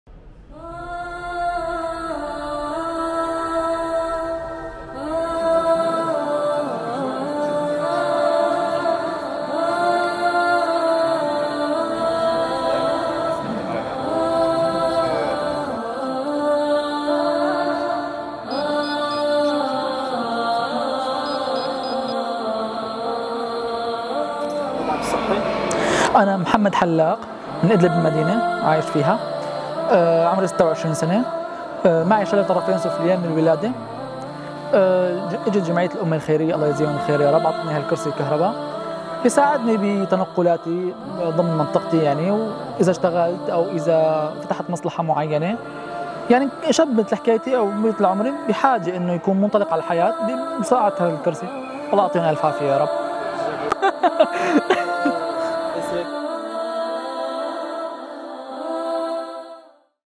A New Wheelchair for Brother Muhammad!
A beautiful video showing how a disabled brother in Syria was provided with a new electric wheelchair. He was one of 32 Muslims helped by Ummah Welfare Trust in the programme alhamdulillah.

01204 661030